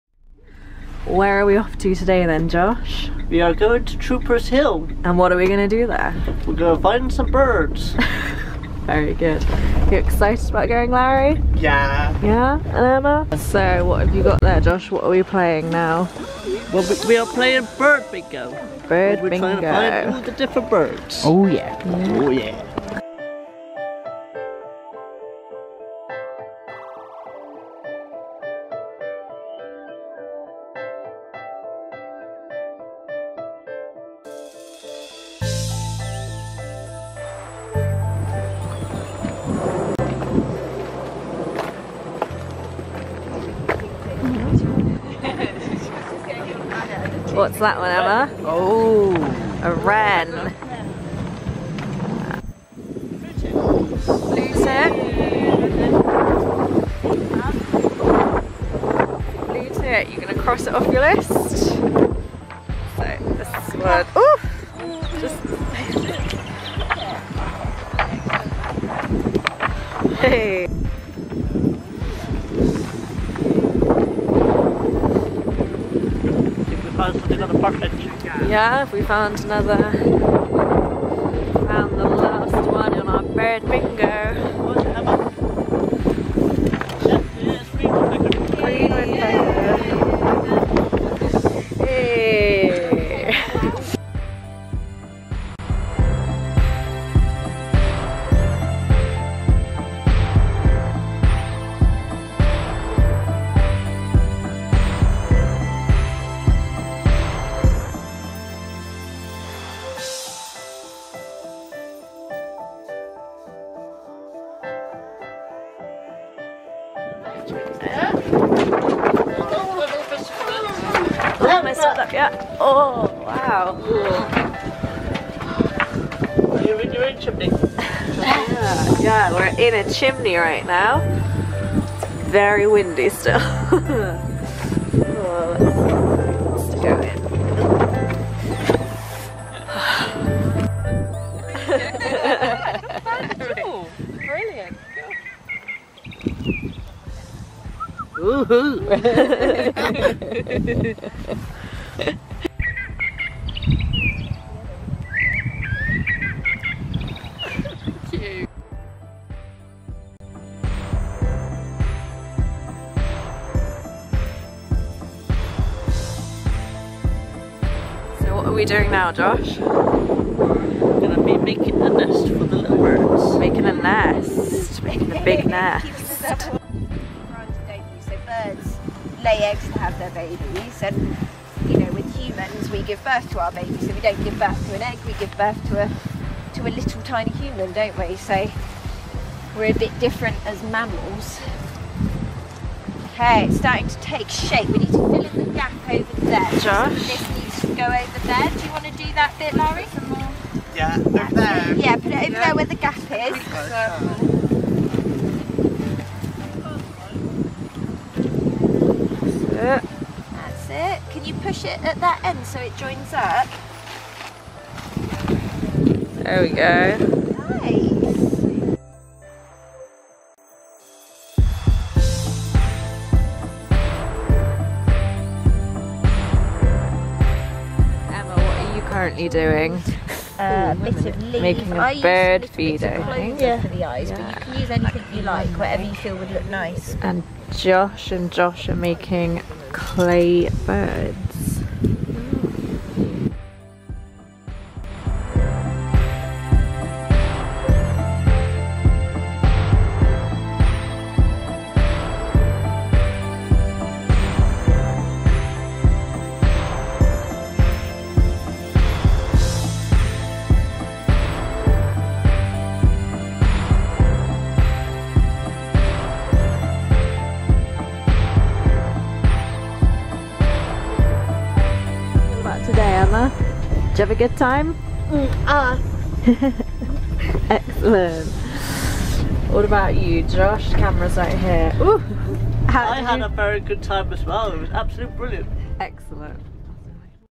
Troopers Hill vlog - Birds
On 25/04/19 a group of us went to Troopers Hill to learn about birds from Natural Learners and Friends of Troopers Hill.
Throughout the day we played Bird Bingo, listened to different bird calls, made a giant bird's nest, and made some crafts.